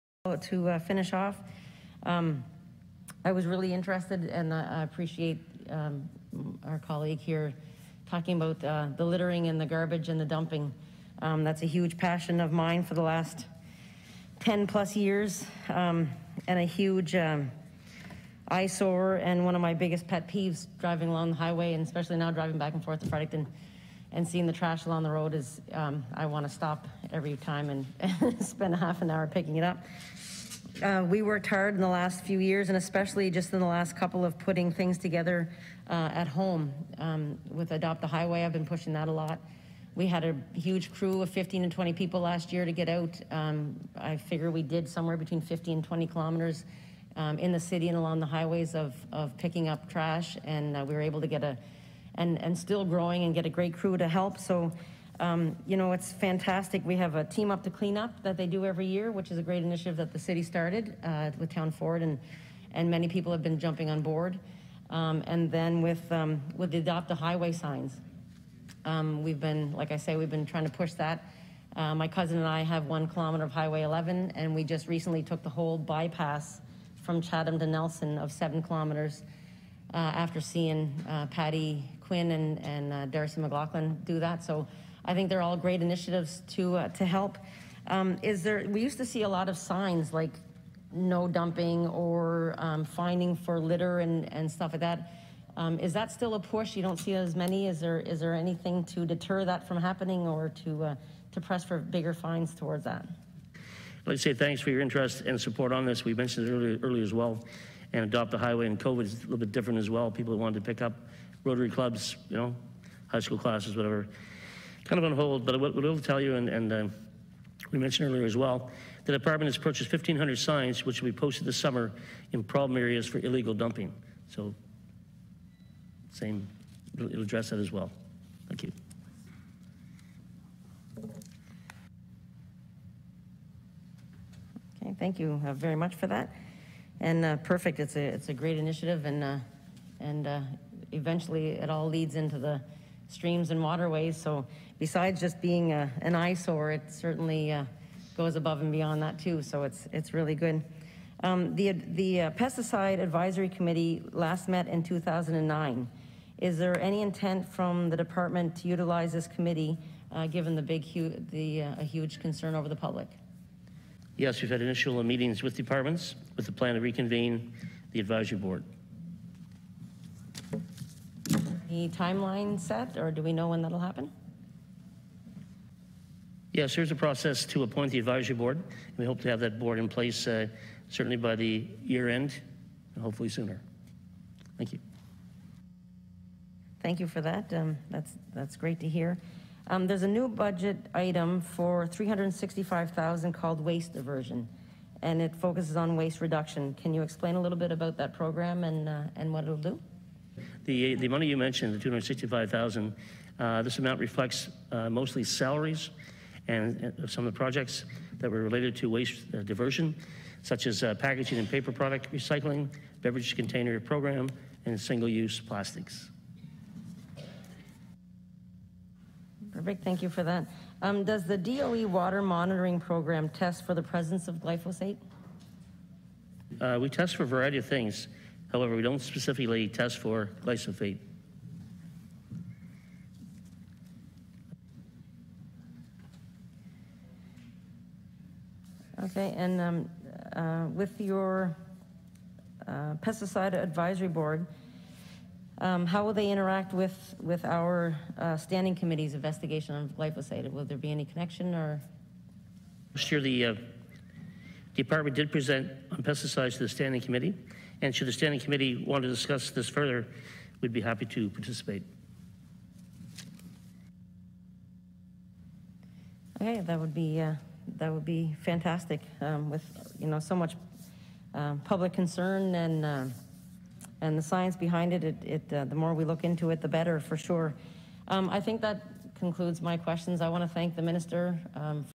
Michelle questions Minister of Environment and Climate Change on glyphosate
Miramichi MLA questions Ministers during Estimates about glyphosate